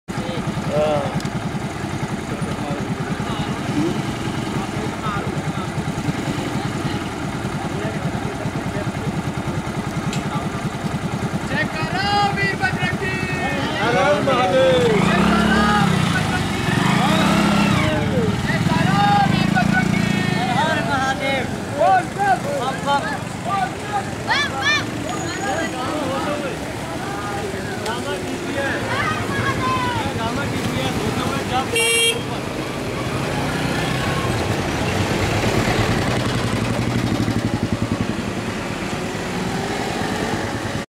Kanwar on skating from Neelkanth , Haridwar to Greater Noida by Rajnikant Thakur and his team
Skating kanwar team lead by Rajnikant Thakur, President, Gautam Budh Nagar Roller Skating Association, fetched Ganga Jal from Haridwar for the second year in a row. They had left for Haridwar on 2/8/2010.INDIA's FIRST SKATER Rajnikant Thakur and Anuj Bhati also reached the HEIGHTS OF NEELKANTH FOR THE SECOND TIME ...Team took a round of City in Greater Noida on 07/08/10. and completed their journey at Gama-I Shiv Mandir "It was a challening task to skate such a long journey...but with with Grace of Lord Shiva ...all was well with all our team. our team is the first in India..." Said Shri Rajnikant Thakur...